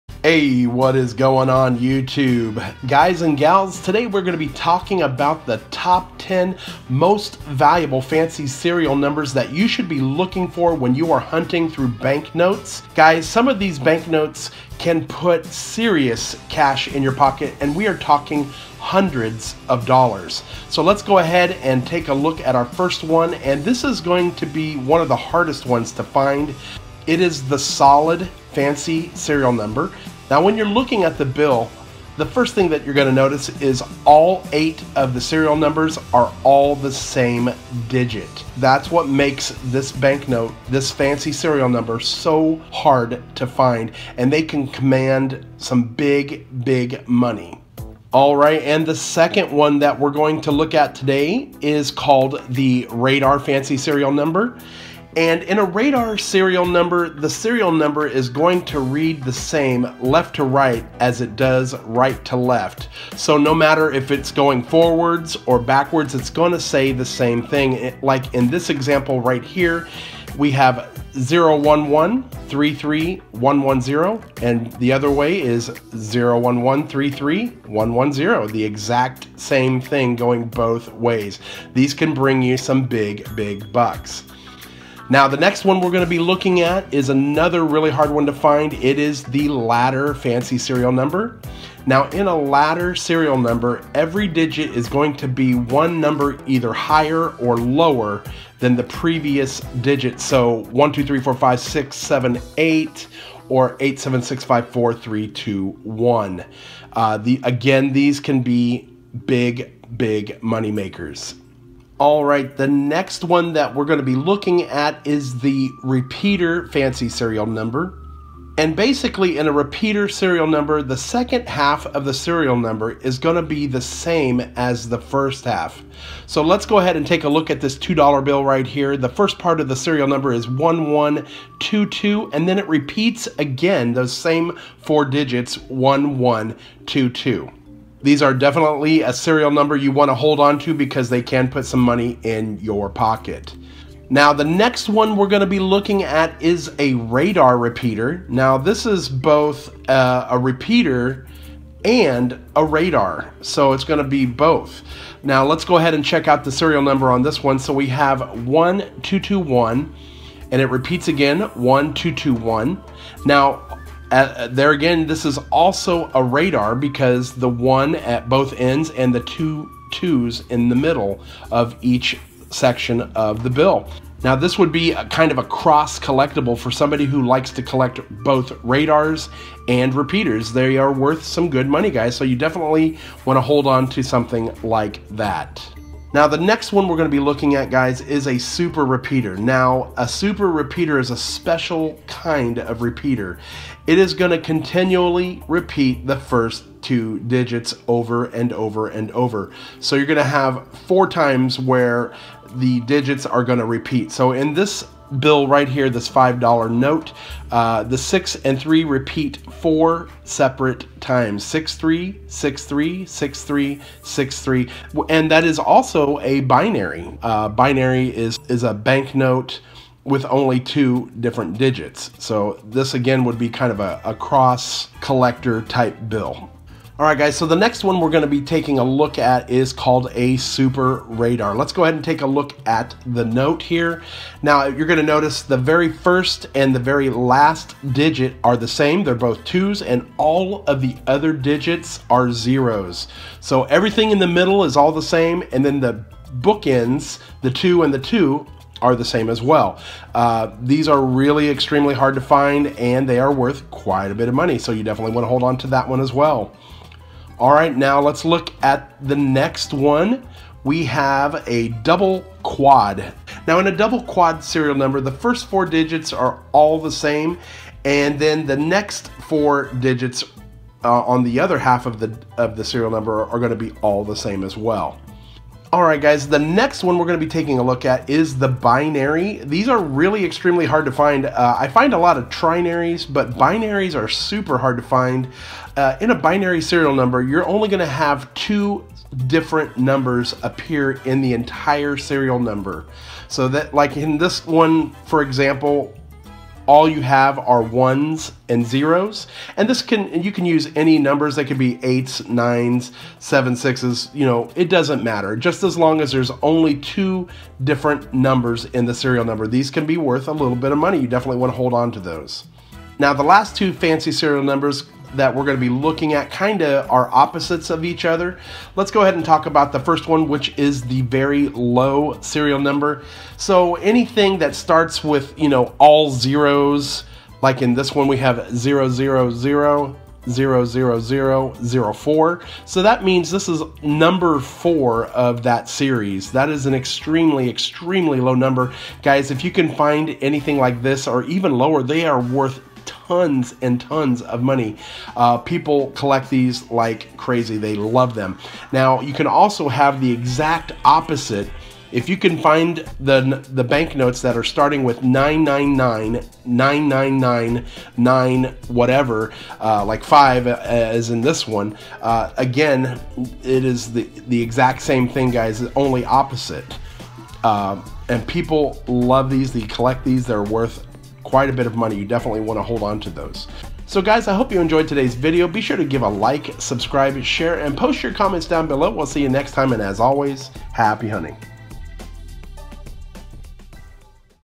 Top 10 Rare And Valuable Fancy Serial Numbers To Search For In Bank Notes
Top 10 Rare & Valuable Serial Numbers to look for when searching Bank Notes!